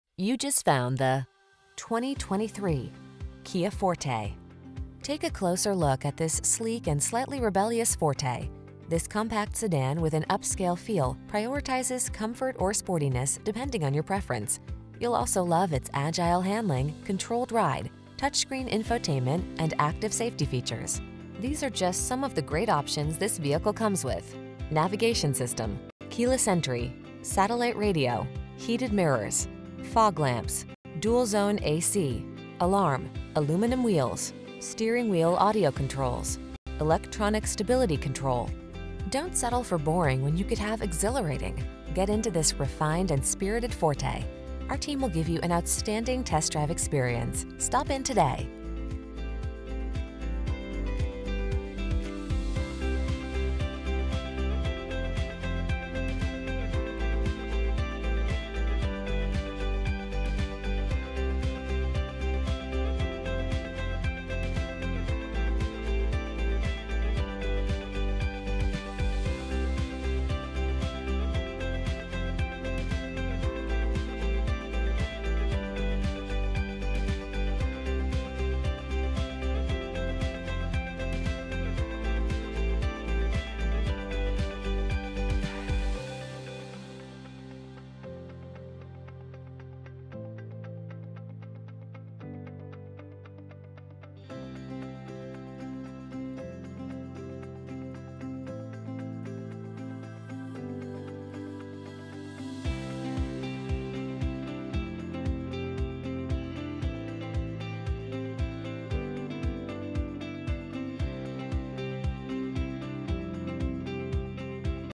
2023 Kia Forte Columbus, Lancaster, Logan, Newark, Chillicothe, OH L23864
Aurora Black New 2023 Kia Forte GT-Line available in Columbus, Ohio at Matt Taylor Kia. Servicing the Lancaster, Logan, Newark, Chillicothe, OH area.

Aurora Black Pearl 2023 Kia Forte GT-Line FWD IVT 2.0L 4-Cylinder MPIbrbrRecent Arrival! 28/39 City/Highway MPG Price includes: $300 - Military Program. Exp. 05/01/2023